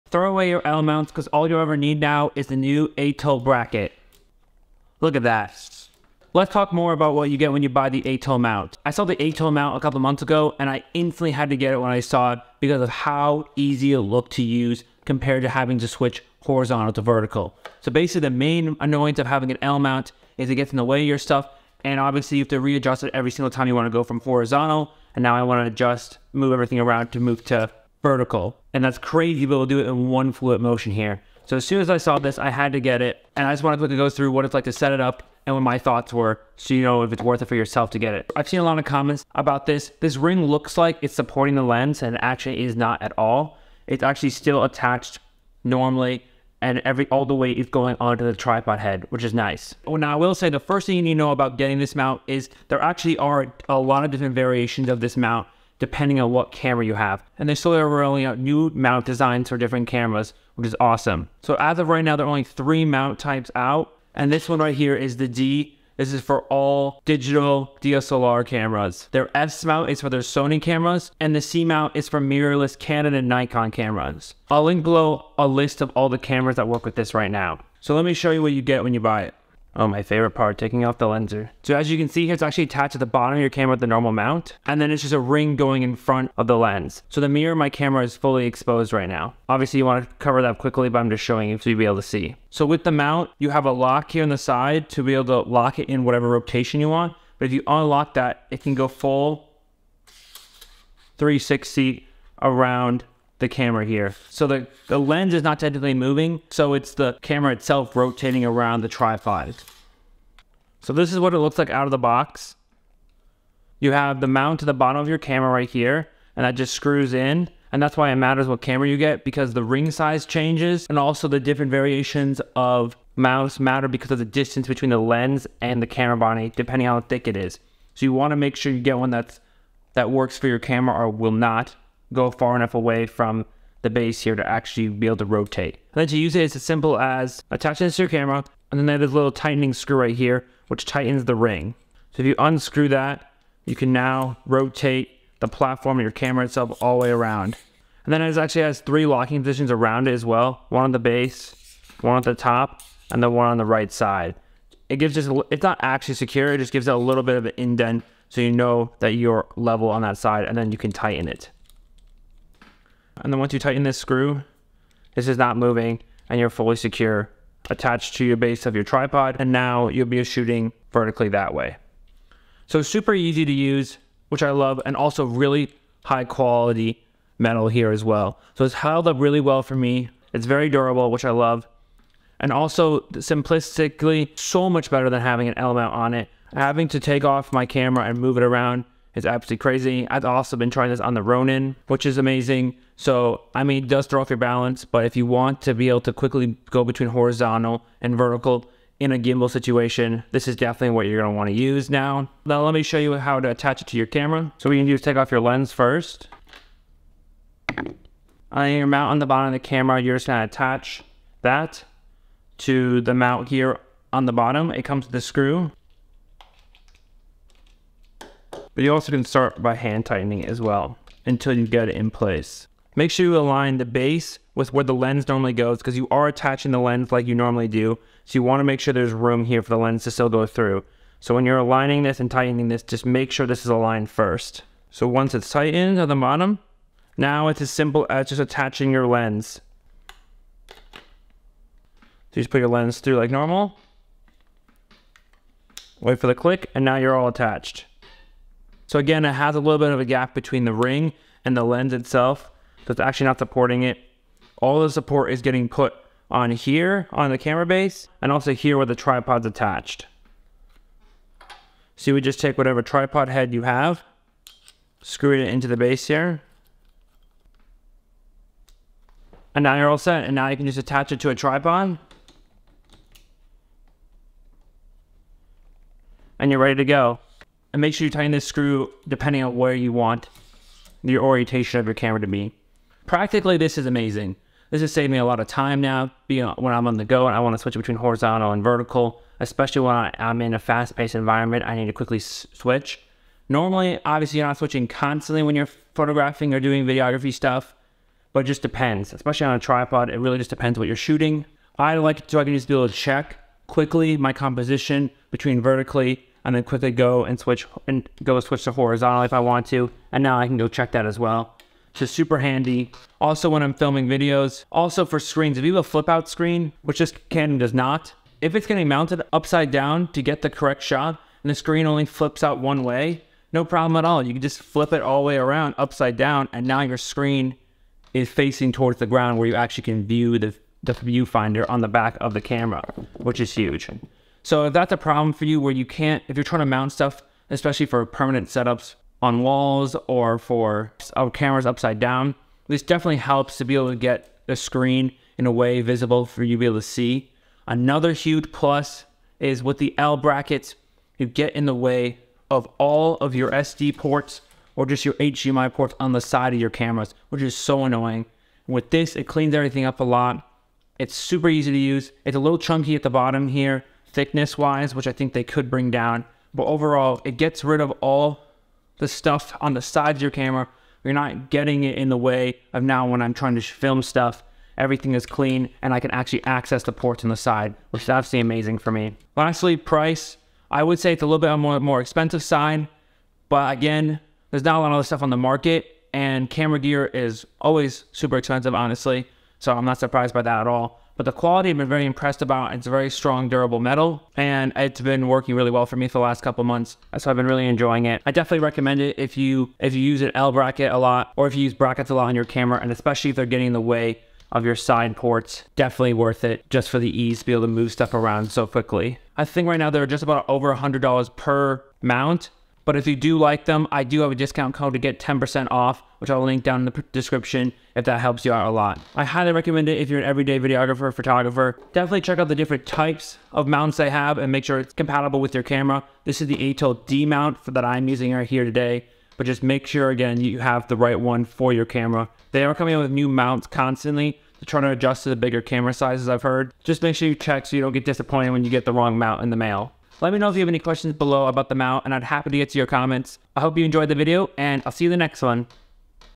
The Only L Bracket You'll Ever Need! - Atoll Mount
In this video, we're going to show you how to use the The Only L Bracket You'll Ever Need! - Atoll Mount!

This bracket is perfect for photographs that need a lot of background removal, like landscapes or nature photos. We'll show you how to use the bracket and give you some tips on how to get the most out of it. After watching this video, you'll have everything you need to take amazing photographs with The Only L Bracket You'll Ever Need! - Atoll Mount! Throw away your L Mounts It's a camera rotate gadget. Like a tripod collar, but way easier and faster and lighter